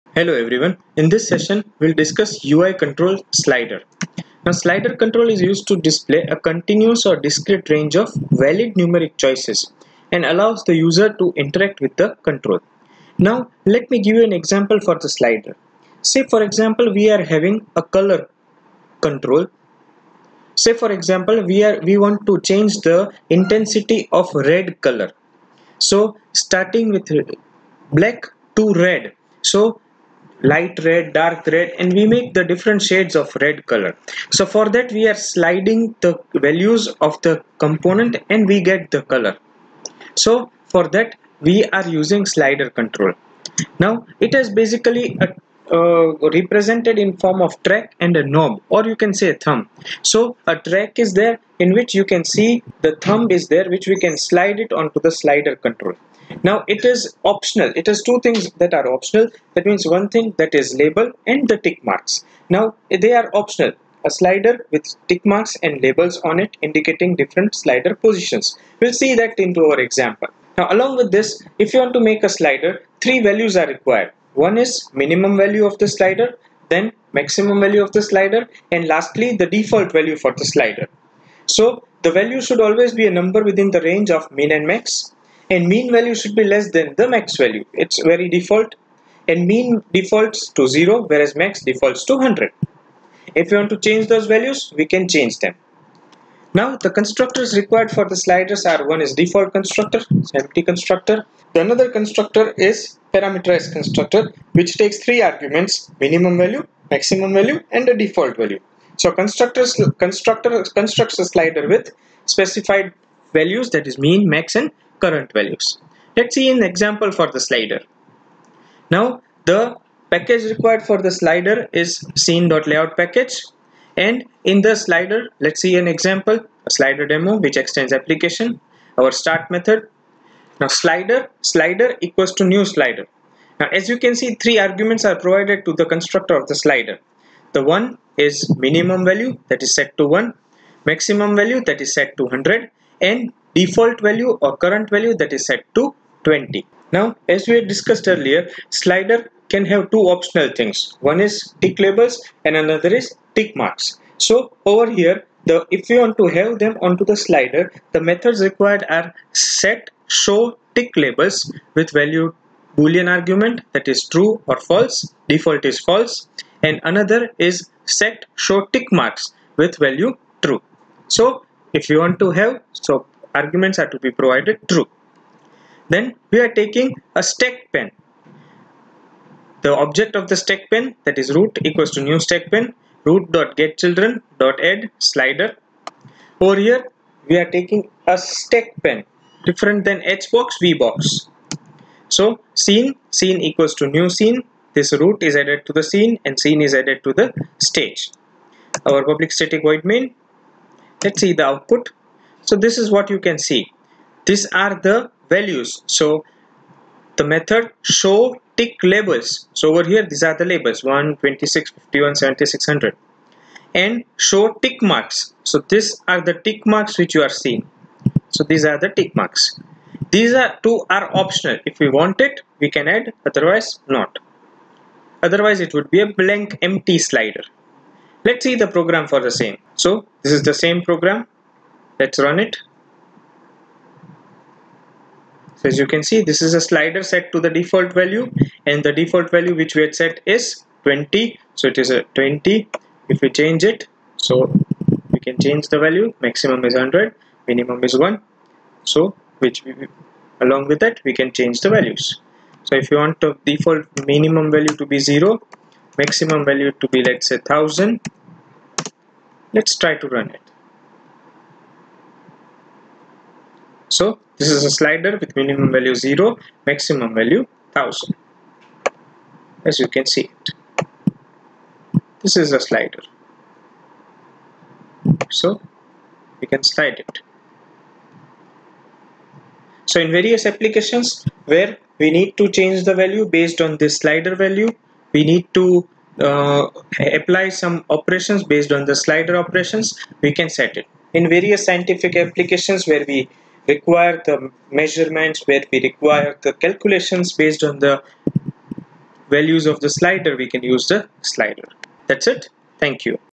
Unit 8: JavaFX - UIControls - Slider
In this session, we will learn Slider - UIControl in JavaFX applications.

This video is recorded by Prof. Hardik A. Doshi ( , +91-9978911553) at the Computer Engineering Department of Darshan Institute of Engineering & Technology, Rajkot.